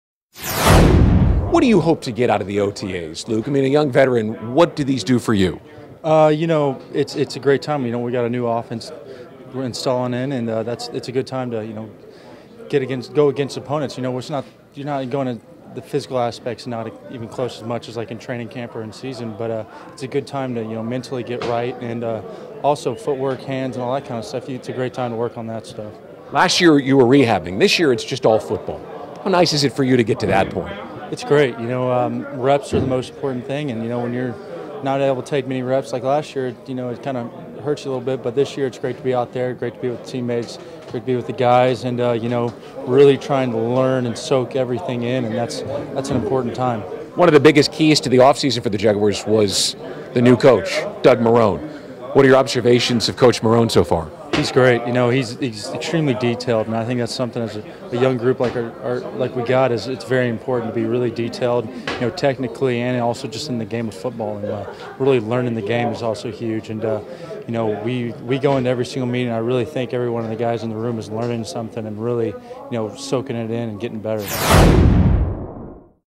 Joeckel: "It's a good time to mentally get right"
Jaguars left tackle Luke Joeckel discusses the start of OTAs with Brian Sexton.